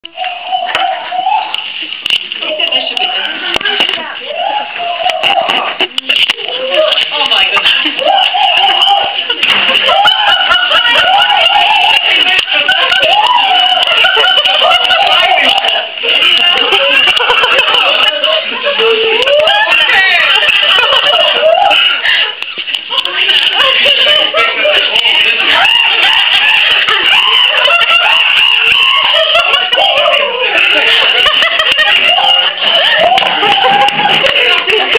Gorilla Bursts into a Class at Canterbury University
A gorilla bursts into an I.T. class at Teachers College, Canterbury University, Christchurch, New Zealand. Thank god for cell phone cameras!